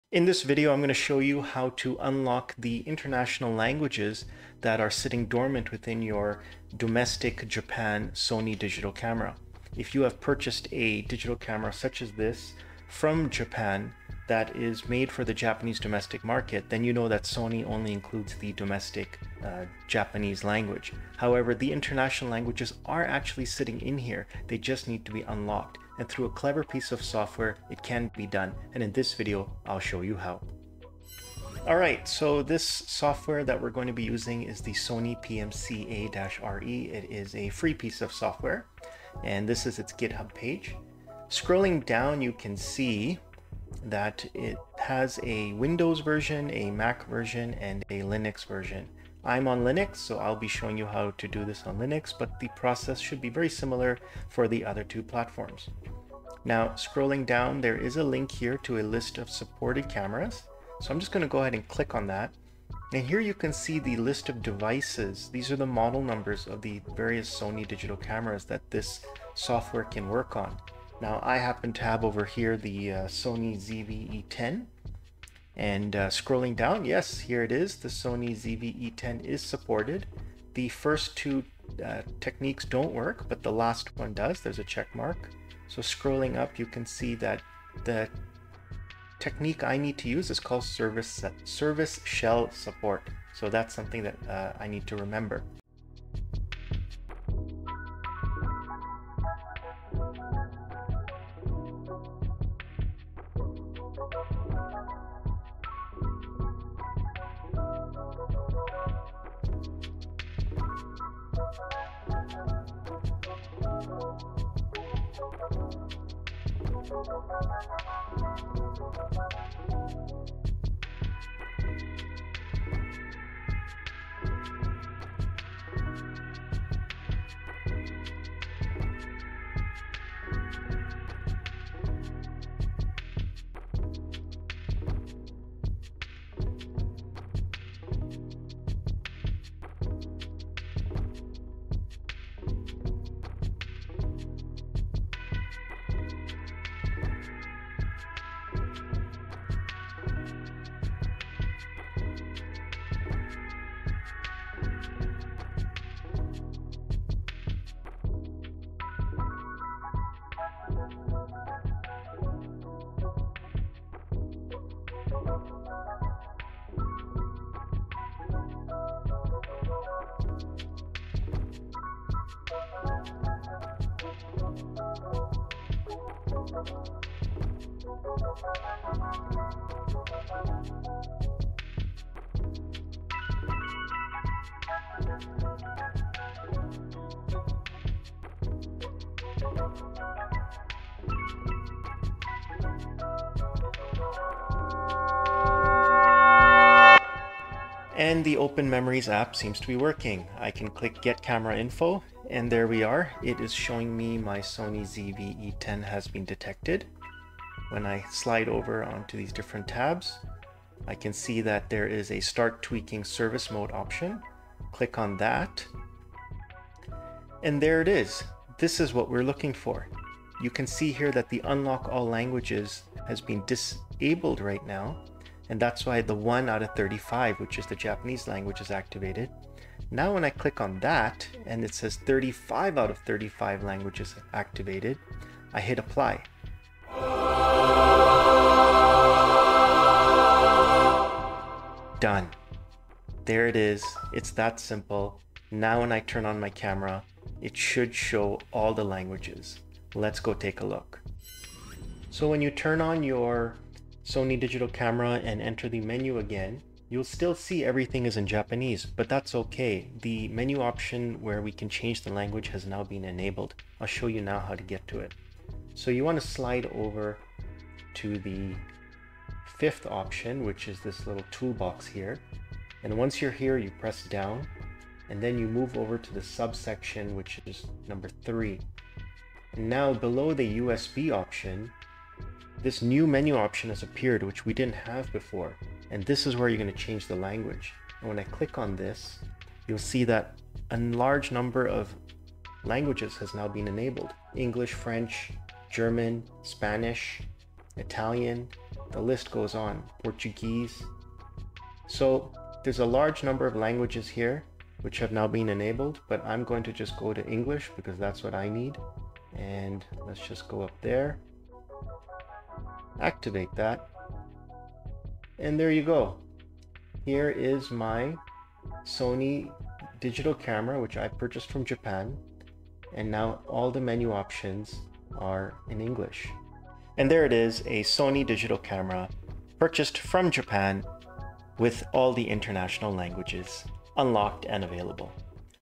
Sony Camera Language Unlock: Quick & Easy Guide!
Tired of the language barrier on your Japan-bought Sony camera? Say no more! In this video, I'll show you how to transform your Sony camera into a global gadget by unlocking all international languages, effortlessly. If you've ever felt stuck with a device that speaks only Japanese, this lifehack is for you.

Using a FREE tool called Sony-PMCA-RE, I navigate its GitHub page to access versions for Windows, Mac, and Linux. As a Linux user myself, I guide you through the process step-by-step, ensuring it's just as easy to follow on other platforms. From checking your camera model's compatibility to activating all languages, I've got you covered:

1. **Verify Compatibility**: Ensure your Sony model is supported.
2. **Download Sony-PMCA-RE**: Choose the version for your operating system.
3. **Unlock Languages**: Follow my lead to unleash 35 languages, including English, French, Spanish, and more.

This no-code solution is a must-try for anyone looking to get the core value out of their Sony camera. Why limit yourself to just one language when you can have the world at your fingertips?

🔥 Check out this video, copy my method, and get FULL ACCESS to all international languages on your Sony camera. No more language barriers – just pure, unadulterated functionality.

LINKS:
Sony-PMCA-RE

CHAPTERS:
0:00 - Unlocking Languages on Sony Cameras
0:32 - Software for Language Change
0:48 - Supported Sony Camera Models
3:17 - Software Implementation Guide
4:09 - Language Setting Adjustment